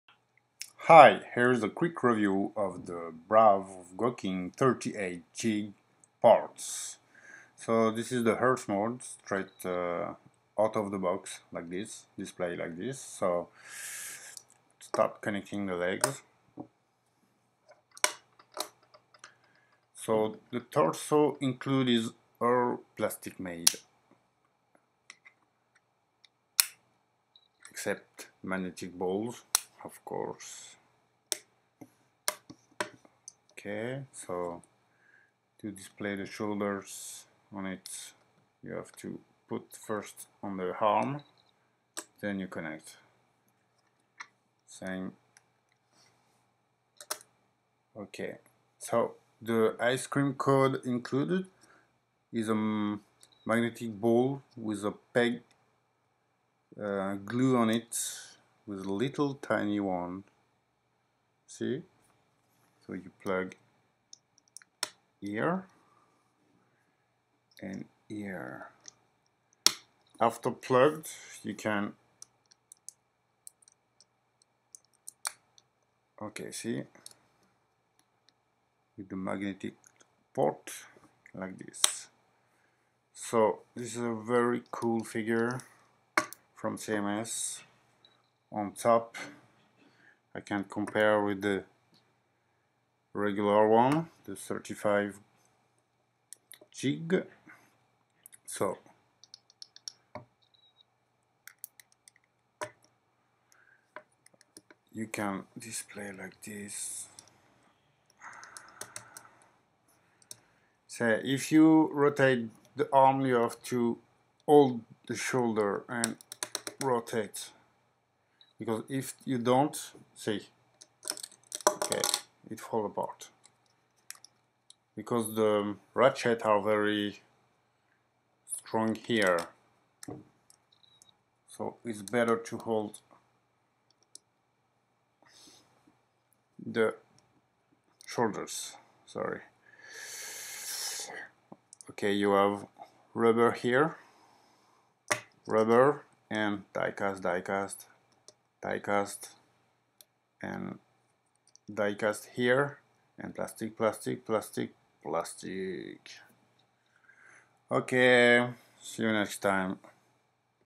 Brave Gohkin 38 - Koutetsu Jeeg Option Parts Set - 1/3
Here is a quick review of the EARTH parts form the Japanese version of the set.